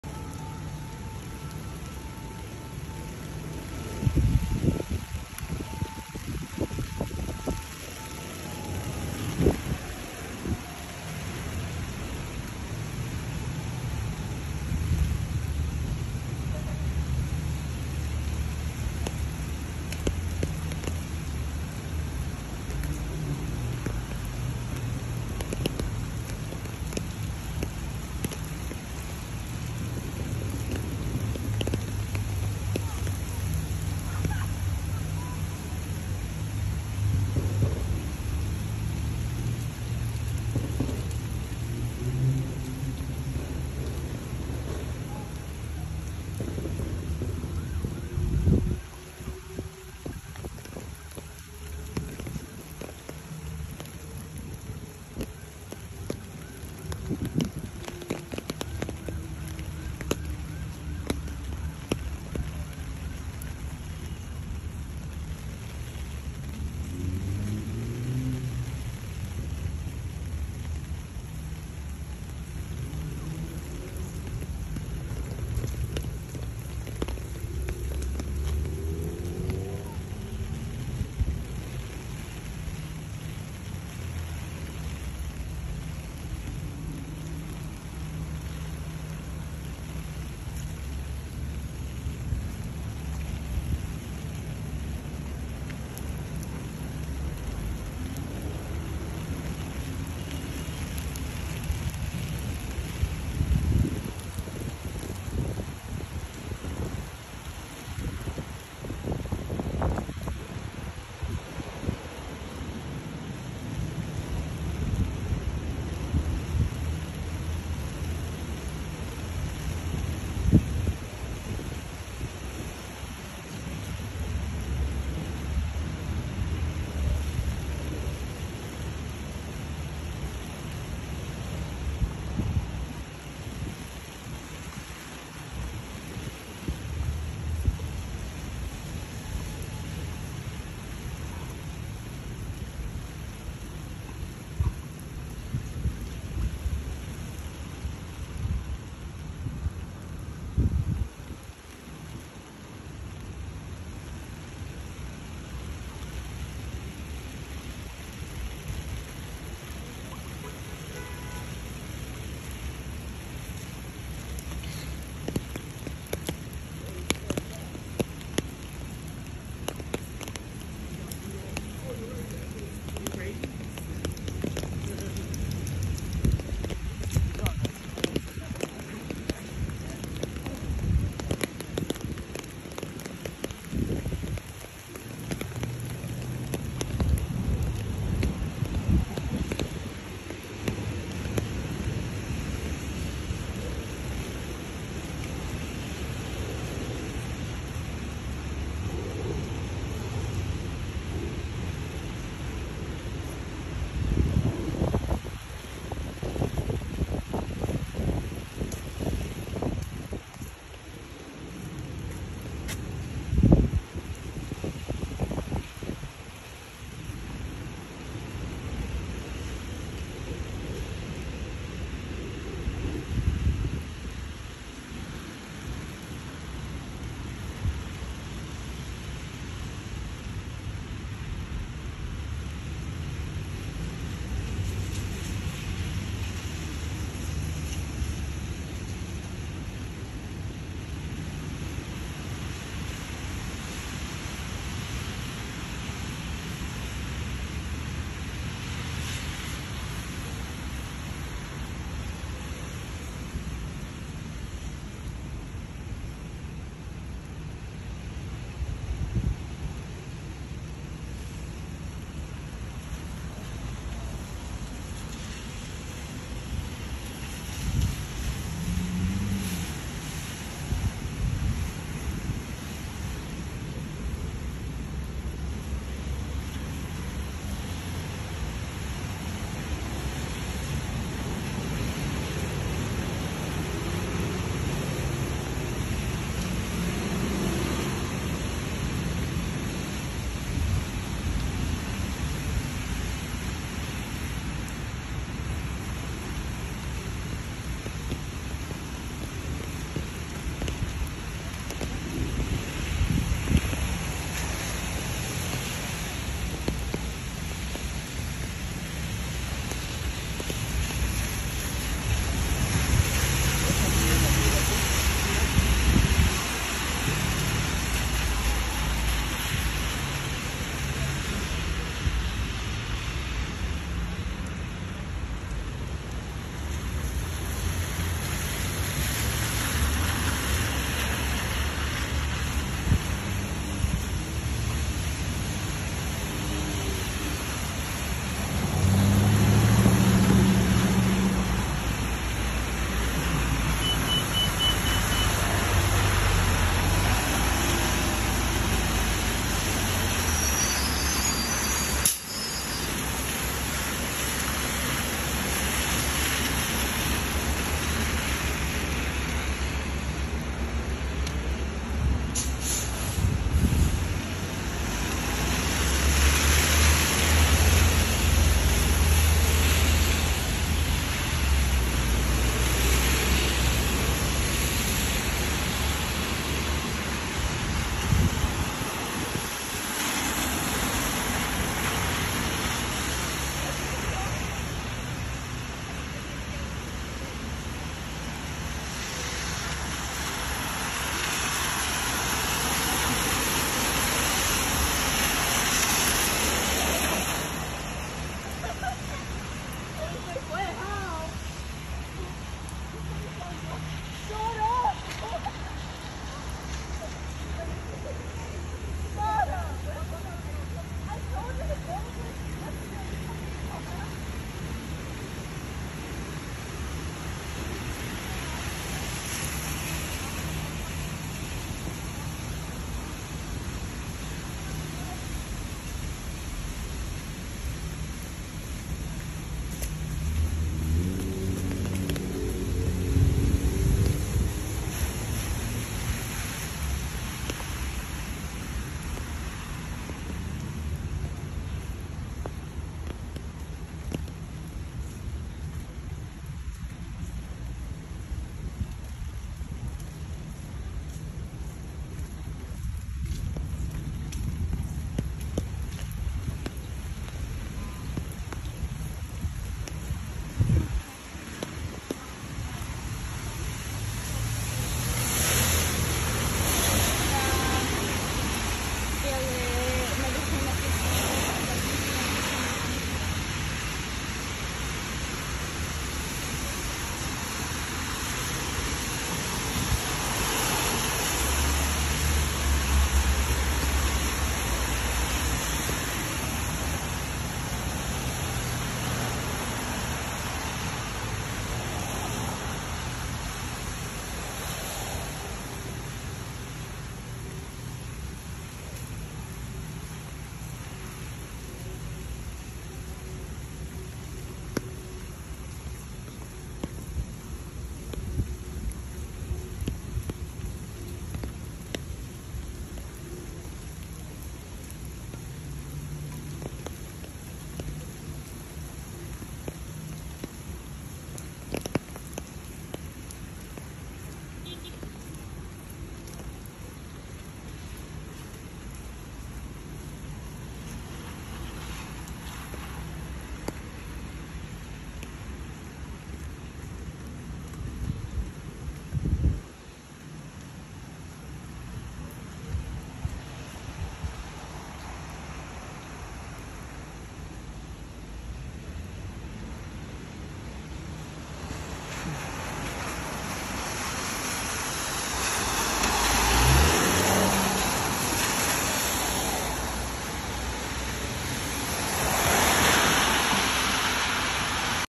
Walking in the rain in Brooklyn, NY
Nice walk in Brooklyn, New York in a rainy day.